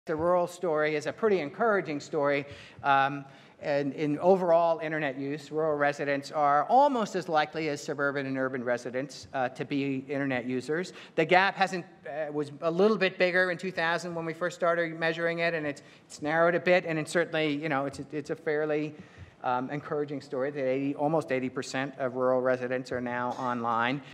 Lee Rainie speaks to the gap between rural usage and suburban or urban usage
Lee Rainie, Director of Internet, Science and Technology at Pew Research Center, explains that internet usage in rural areas is only narrowly behind suburban and urban usage.